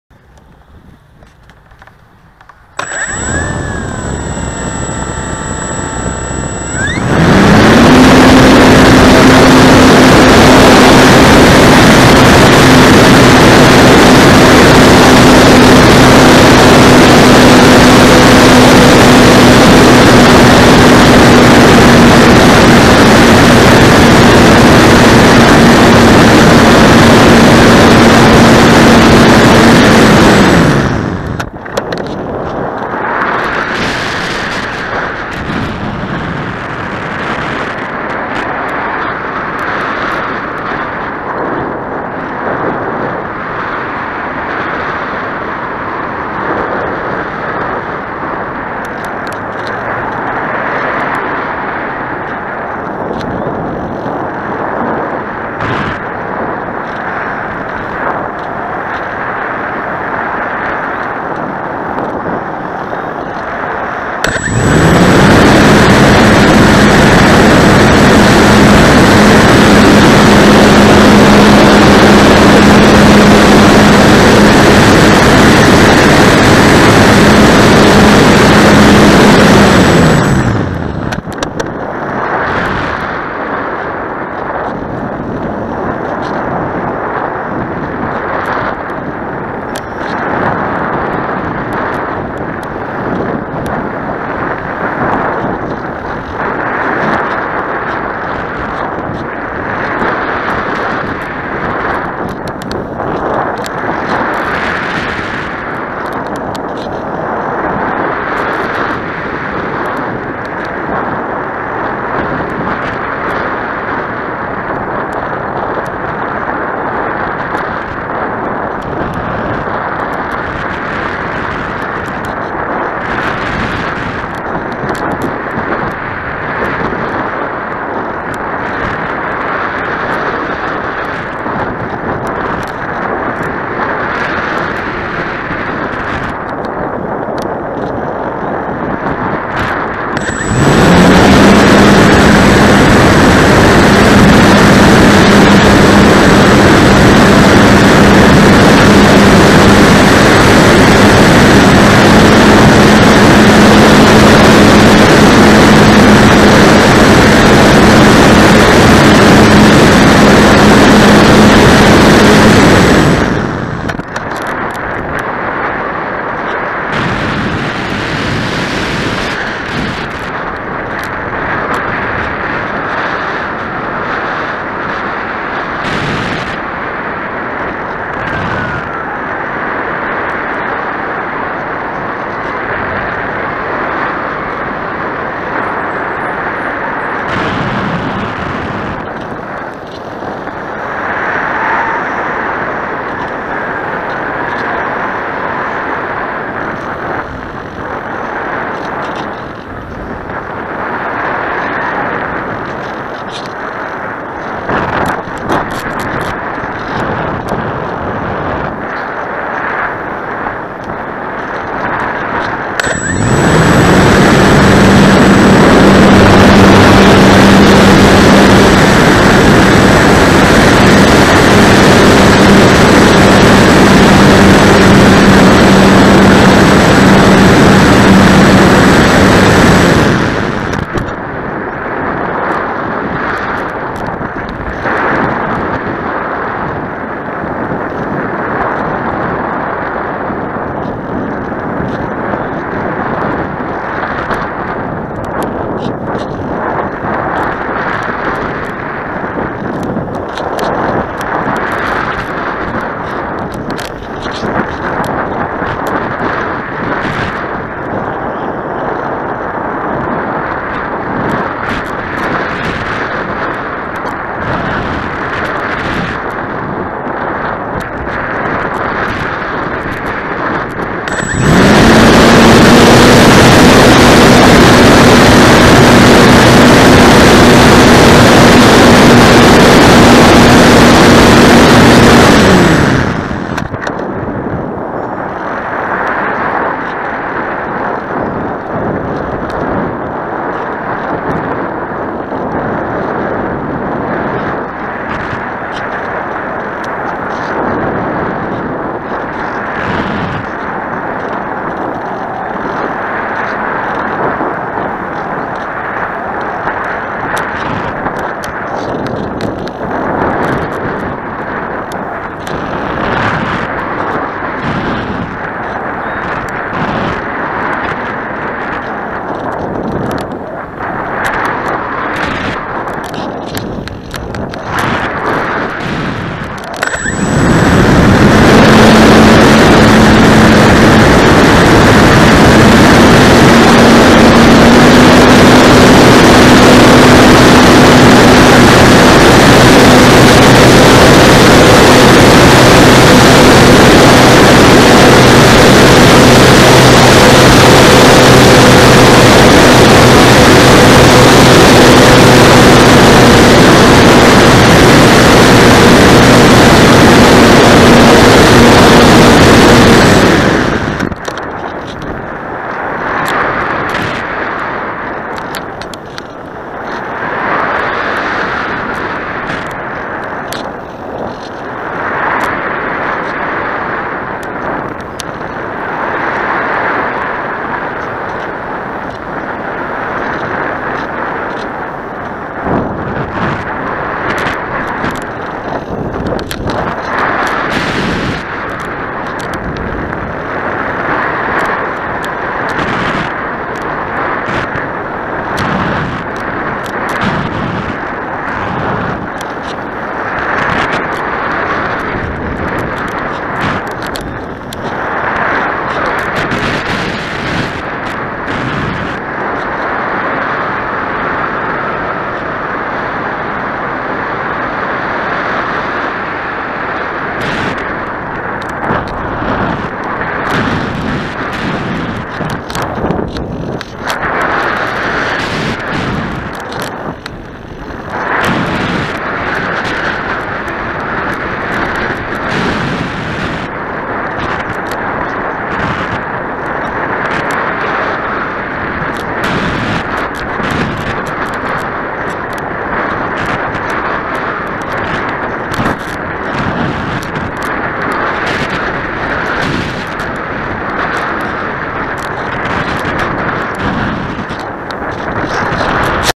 Radian, On Board, Aaron Bessant Park, Pier Park, 18Jul14
Quick afternoon 1715, fly around Aaron Bessant Park Panama City Beach adjacent to Pier Park.
When I watch these videos I like using the pause button to check out the view or download it and slow the speed of the video down.
The winds were picking up during this flight I kept the altitude down a little for that reason.